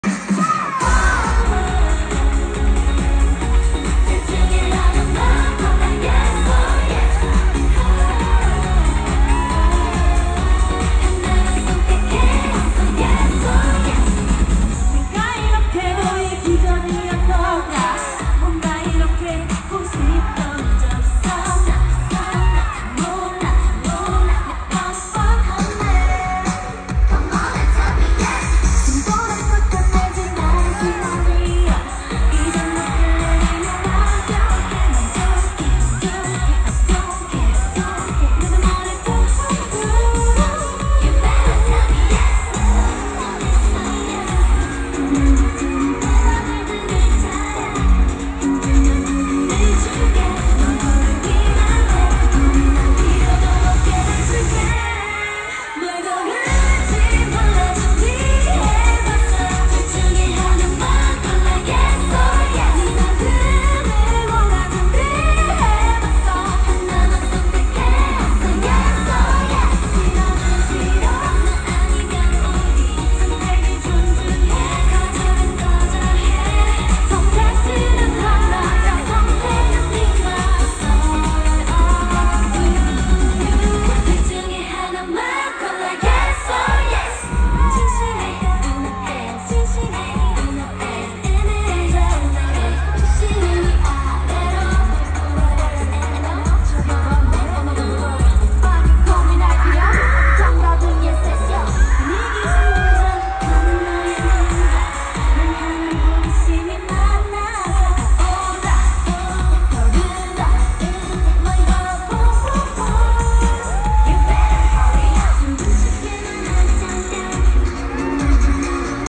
Twice World Tour 2022
My girlfriend and I went to the Twice World Your in California last year!